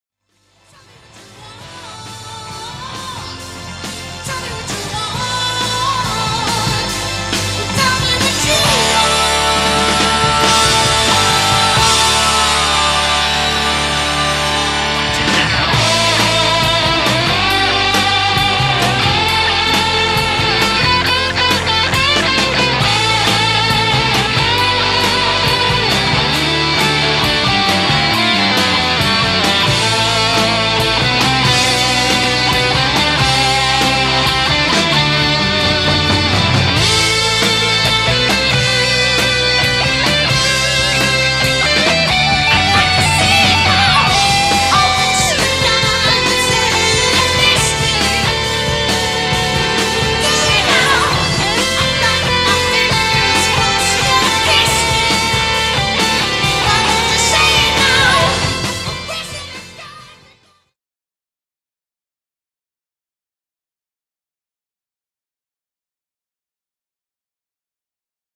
Hondo II Guitar I 1982 I Test I Made in Japán
Guiatar Solo Cover: MrNono5150
G u i t a r   S o u n d:
*Hondo II Guitar (1982) Made in Japán
*Line 6 FLOOR POD
*Mackie Audio MR5
 *VONYX - VMM-K602